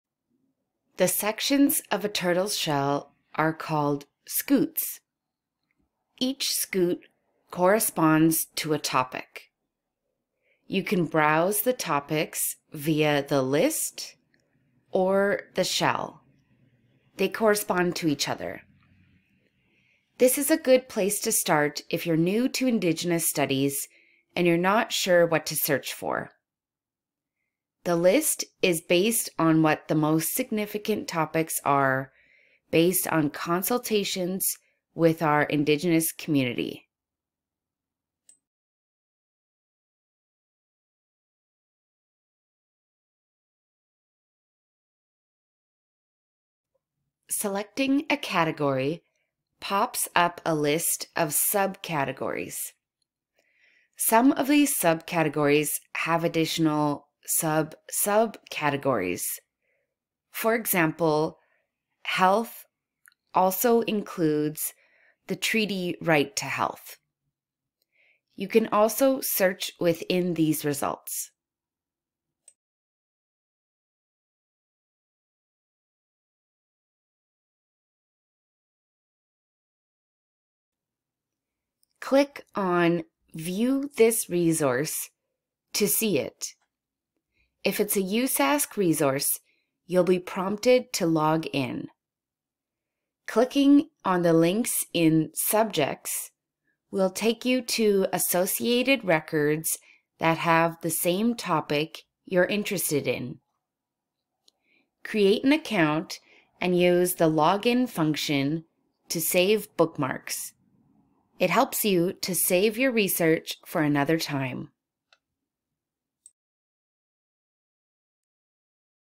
I-Portal: Browse the Turtle
Learn about how to use the Turtle interface on the Indigenous Studies Portal (I-Portal) to browse the different subjects available. Check out the I-Portal at iportal.usask.ca